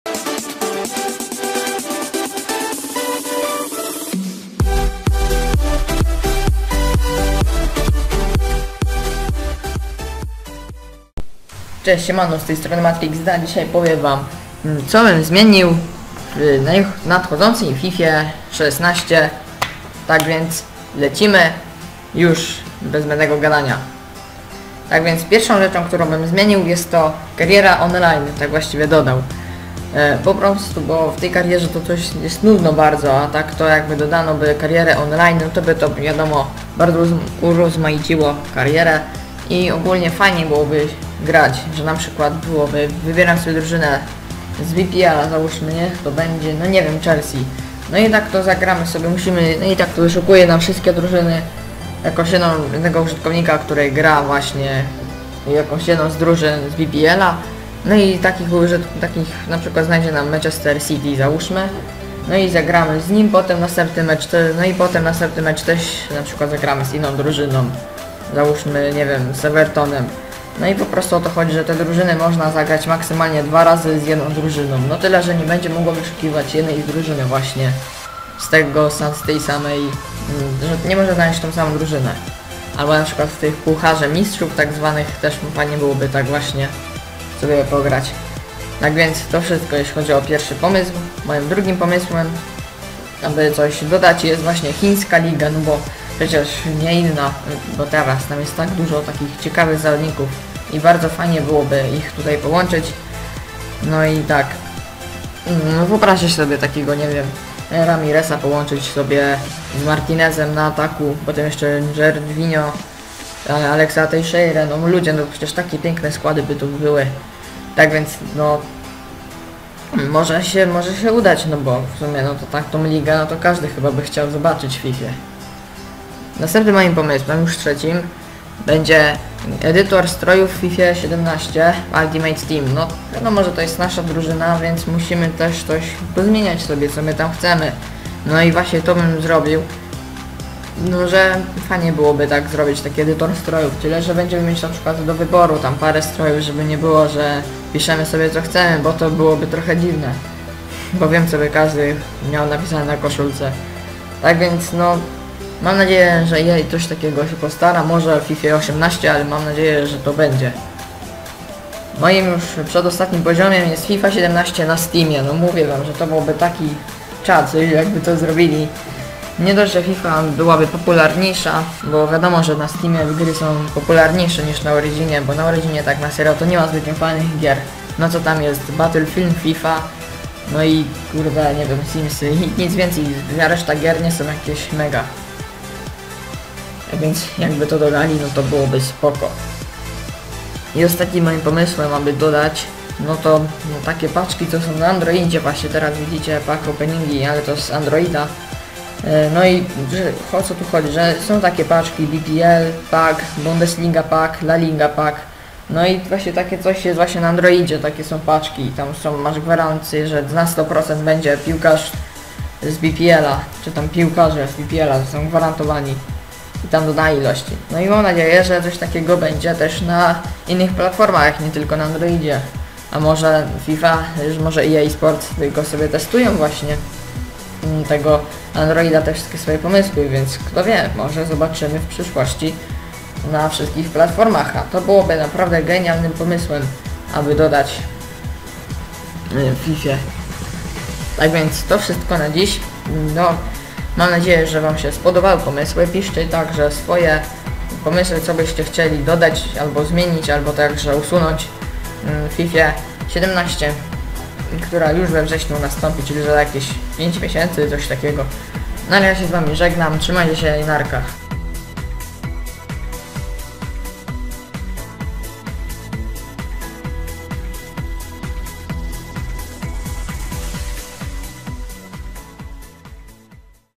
5 RZECZY, KTÓRE DODAŁBYM DO FIFY 17
Siemka z tej strony MatrixD, dzisiaj zobaczycie 5 rzeczy, które dodałbym do FIFY 17, mam nadzieje że zostaną wkrótce dodane :D
Piszcie także swoje propozycje na dole :D
Po obejrzeniu filmu:
•Zostaw Like pod filmem :)
•Napisz Komentarz :)
• Dzięki :D